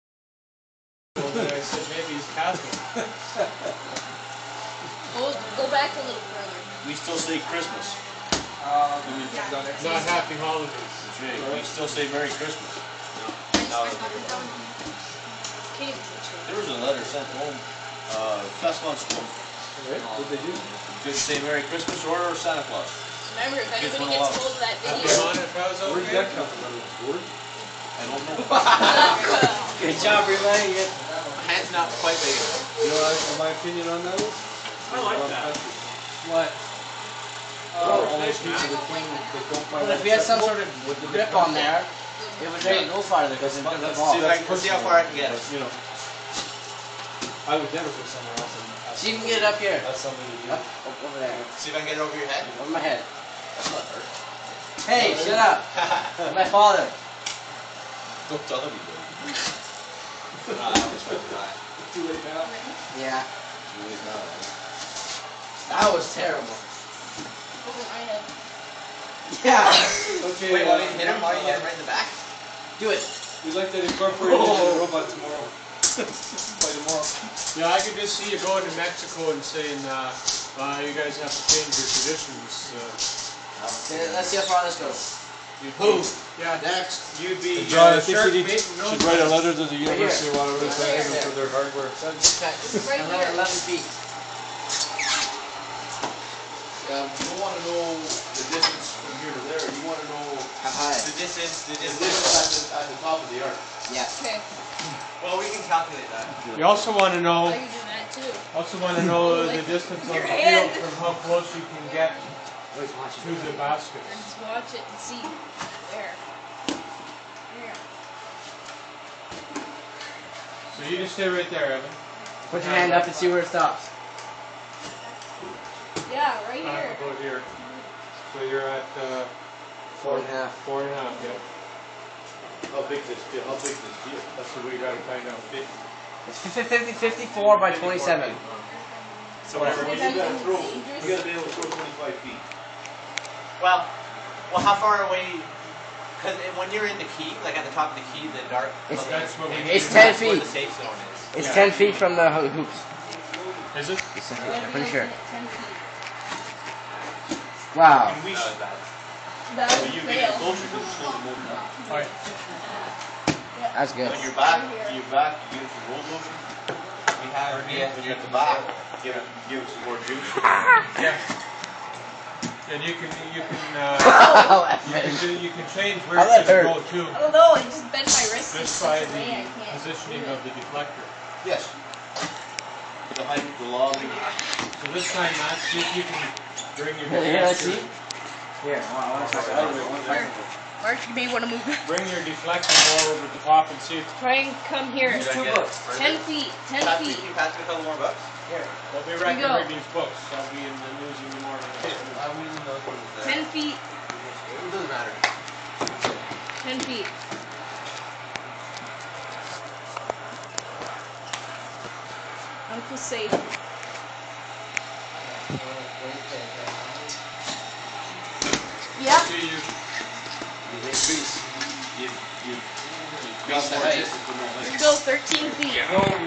Ball shooter, proof of concept.
Just took an old motor and drum setup (that I grabbed from the university in a pile labeled "junk") to fling some regulation FIRST Robotics Competition game pieces for the 2012 game Rebound Rumble.